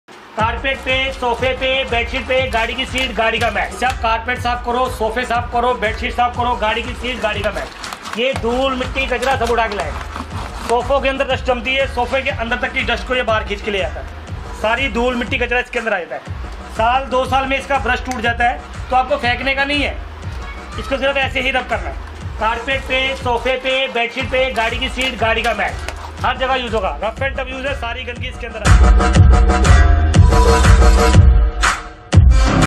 New Advance Reusable Handheld Crumb Sweeper
ALL IN ON New Advance Reusable Handheld Crumb Sweeper
High Performance: Rolling to clean, easy to remove all kinds of dirt such as nutshell, paper scraps, etc. The brush head covers a large area, high cleaning effect.